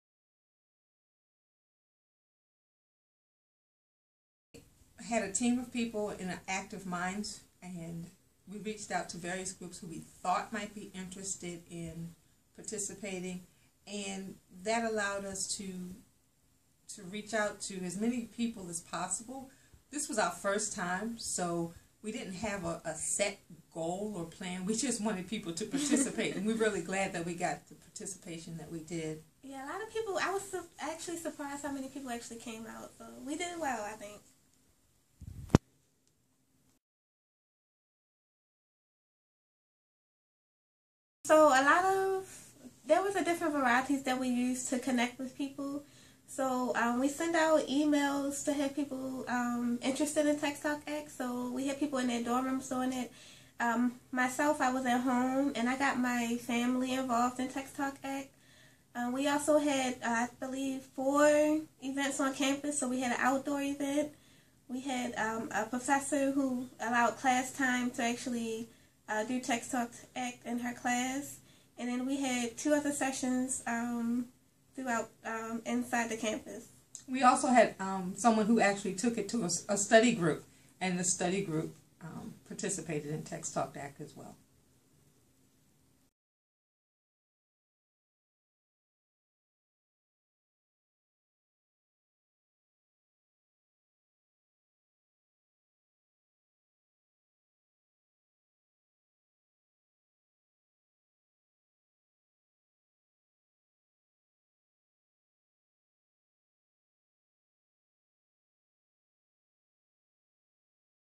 TextTalkAct - Making a Plan
The Coppin State University Active Minds chapter discusses how they formed their plan and then how they helped to spread the word about TextTalkAct on their campus. Participate in TextTalkAct on 4/14/2015 and 5/7/2015 by texting "START" to "89800".